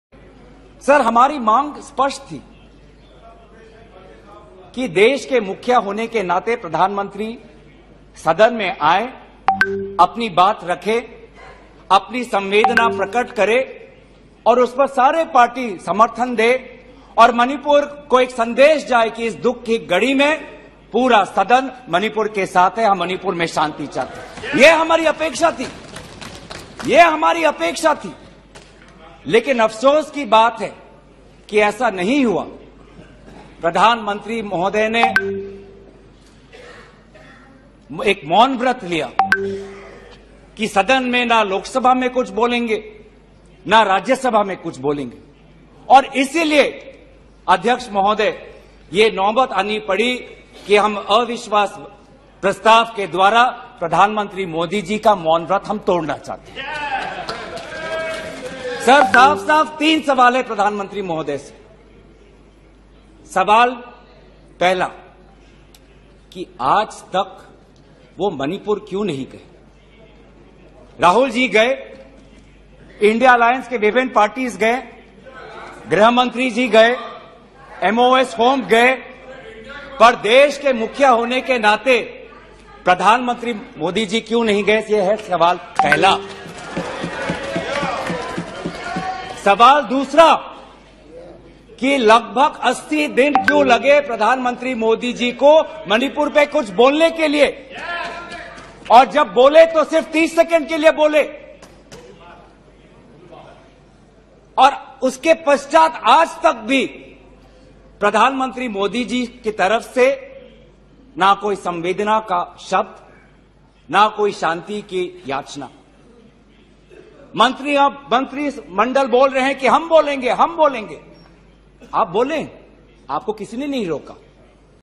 NO-CONFIDENCE MOTION TO BREAK PM'S 'MAUN VRAT' ON MANIPUR: GAURAV GOGOI
Congress MP Gaurav Gogoi says, PM took a 'maun vrat' to not speak in the Parliament. He asks PM why did he not visit Manipur to date, why did it take almost 80 days to finally speak on Manipur and why has the PM not sacked the Manipur CM so far.